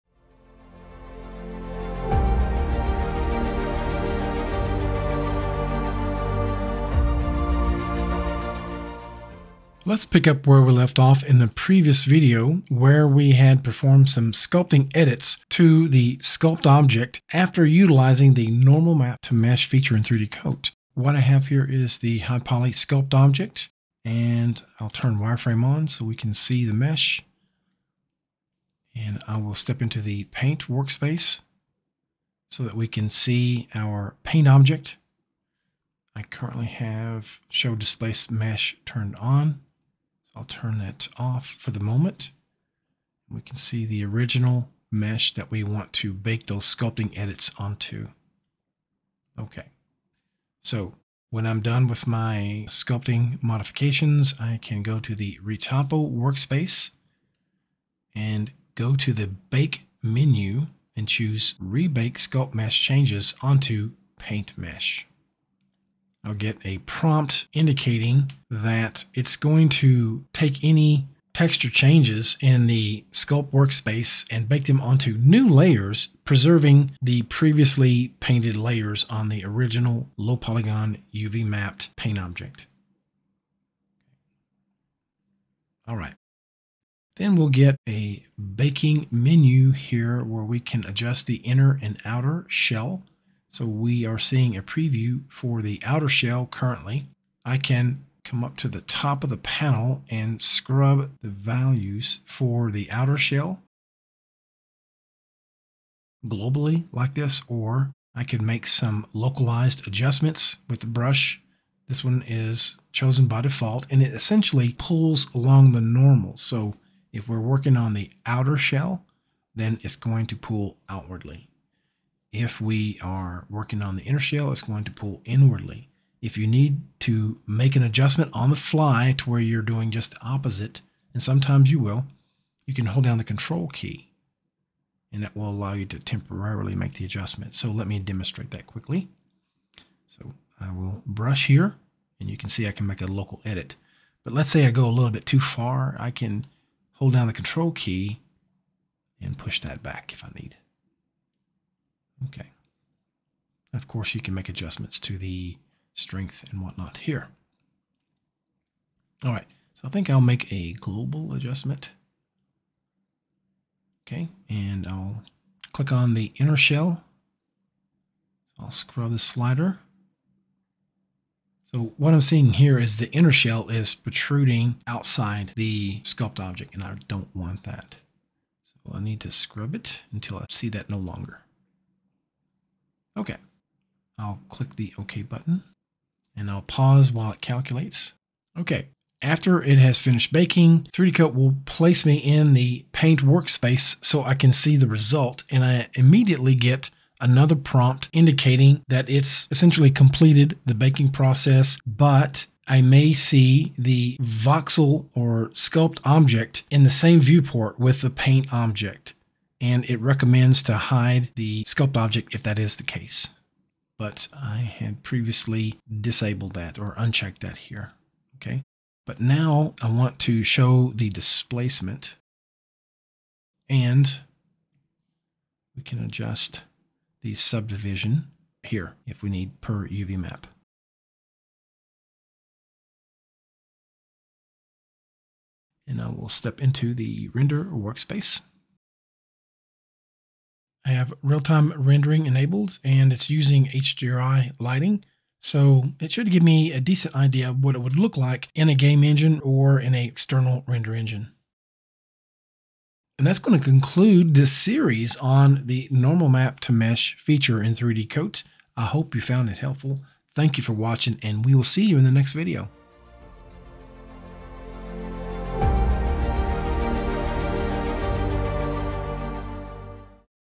Normal Map to Mesh Pt 3
This video continues the demonstration of the Normal Map to Mesh feature by baking the Sculpting edits (on the High Poly Mesh) back onto the original, low-poly Paint Mesh.